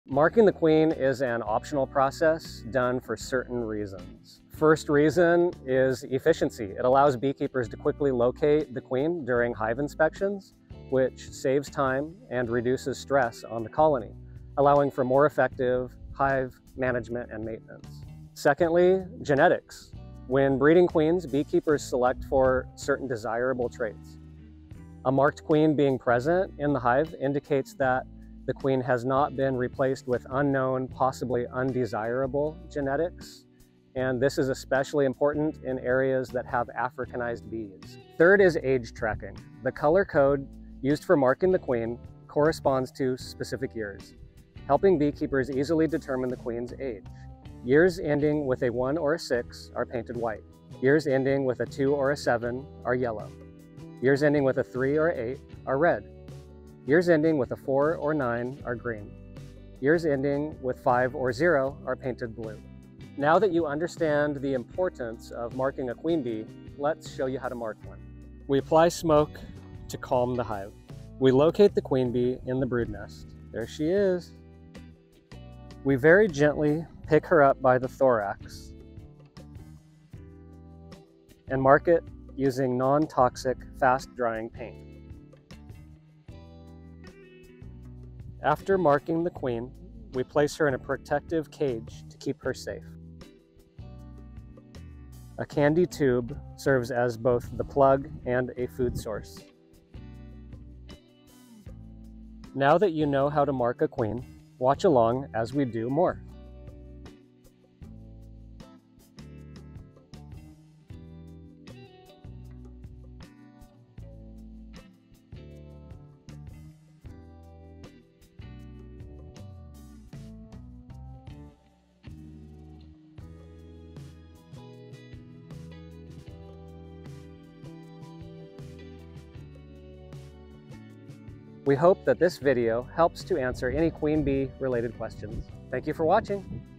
How To Mark A Queen Bee! 👑🐝
Here's an in-depth look on why and how to mark a queen bee!